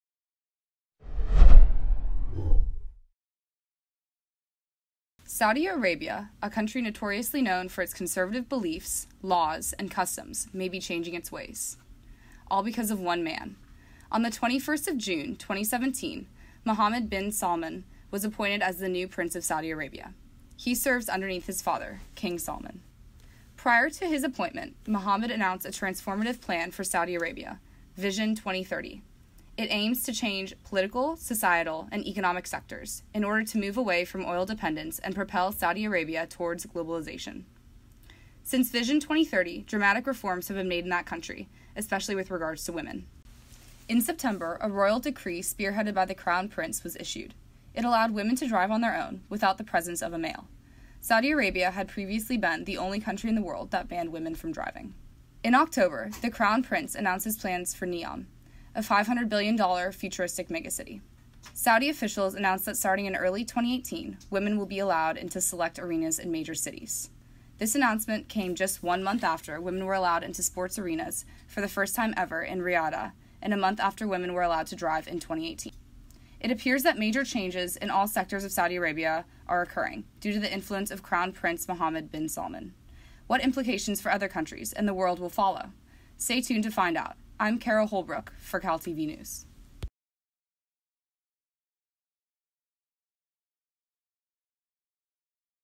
Saudi Arabia Enacts Reforms
Learn about recent reforms spearheaded by Crown Prince Mohammed Bin Salman in Saudi Arabia, including women's freedom to drive, NEOM megacity initiative, and Saudi Arabia's overall push to globalization. CalTV News' Kara Holbrook reports.